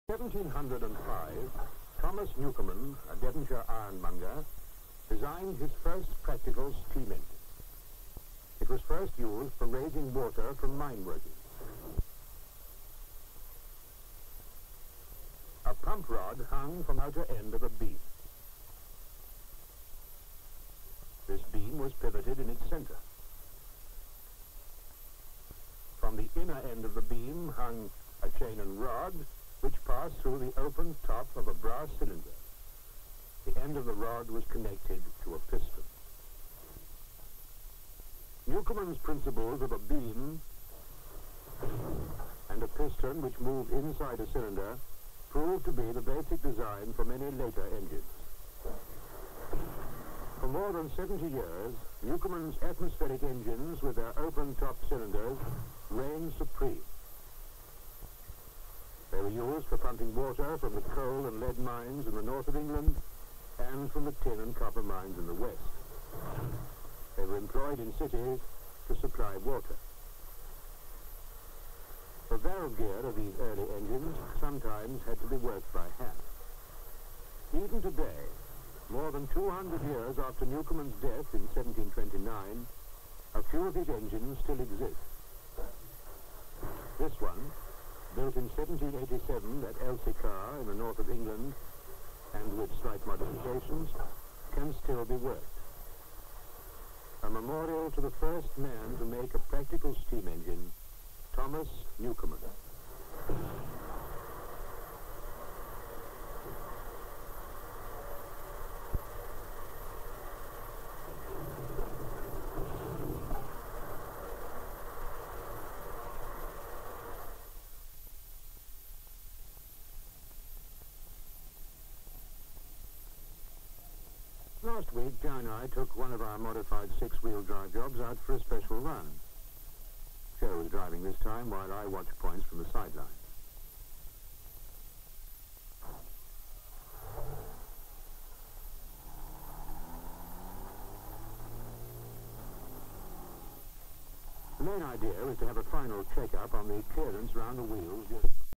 Newcomen's Atmospheric Engine, 1950s - Film 1004463
Newcomen atmospheric engine 1950's
Sketches of steam engined designed by Newcomen. Piston moving in the cyclinder pumping water. Man operating the atmospheric engine by hand pump; an open fire burns in a hearth behind him. View of the pivoting beam above. 1787 date carved into lintel above doorway. View through the door of the engine in operation at Elsecar Heritage Centre. Overhead view of the moving piston. Close up of beam. View of the brick building housing the engine, as viewed from the exterior.
An unrelated clip now appears, where two men stand beside a large six wheeler truck, looking at a map. Driver throws away his cigarette and climbs into the cab. View of him behind the wheel as he starts the engine. He begins to drive on a rough off road surface before the film abruptly ends.